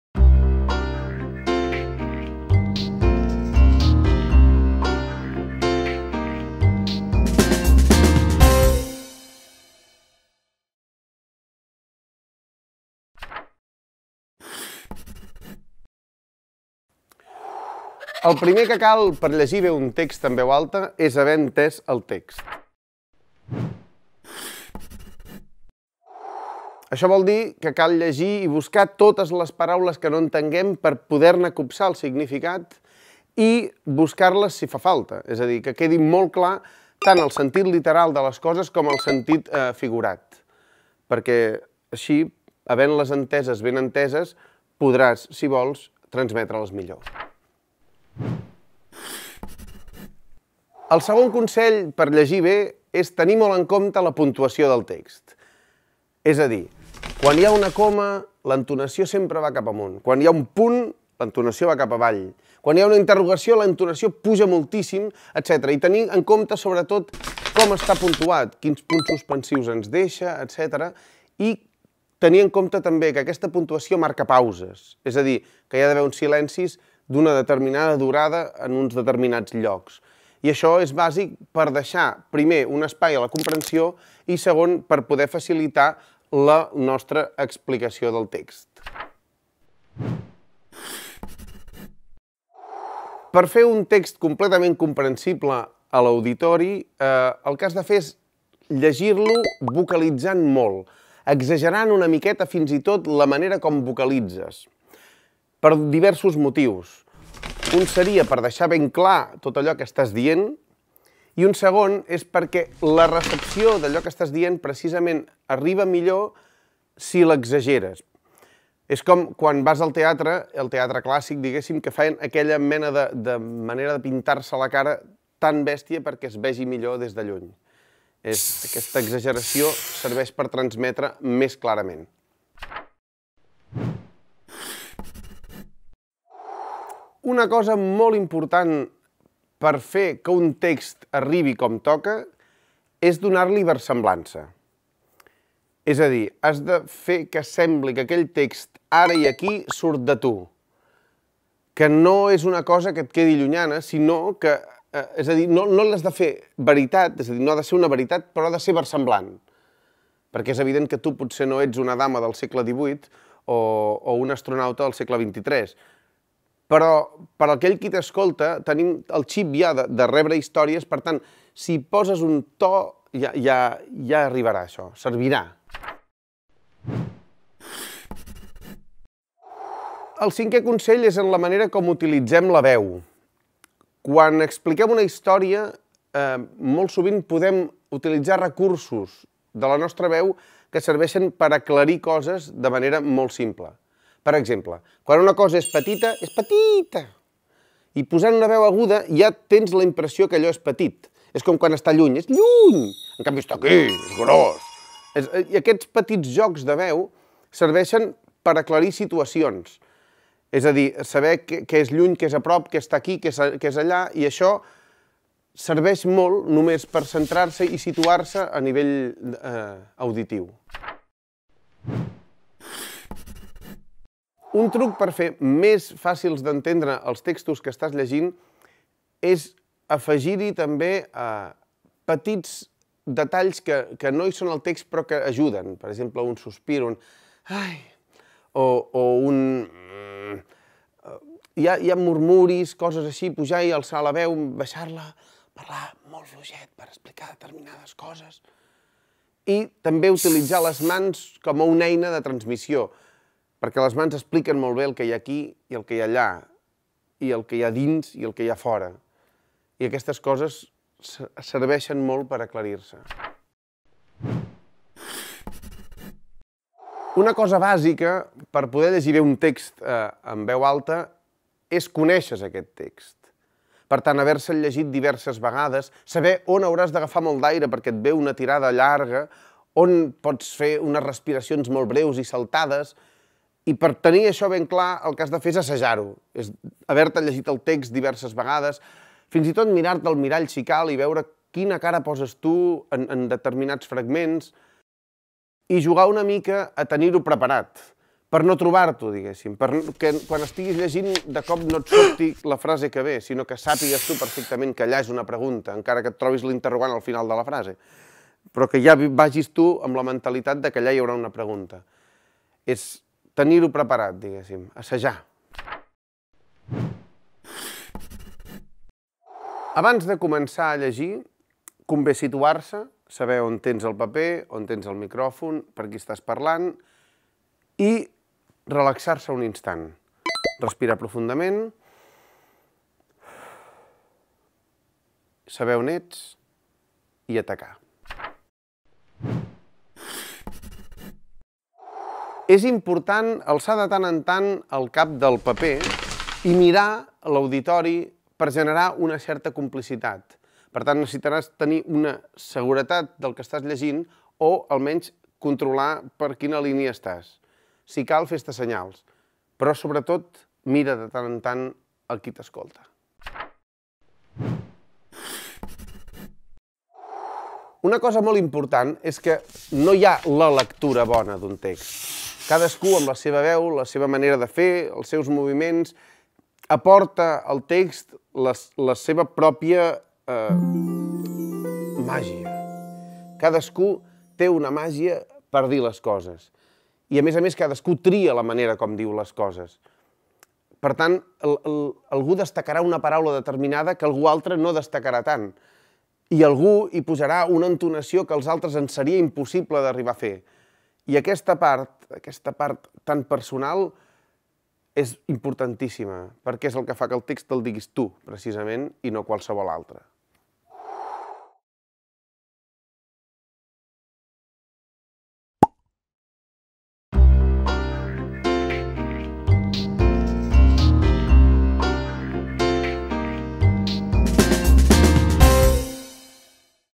10 consells per a una bona lectura en veu alta - Josep Pedrals
Llegir en veu alta és una activitat molt recomanable per treballar la llengua oral. En aquest taller hi trobaràs 10 consells per fer una bona lectura en veu alta a l'aula.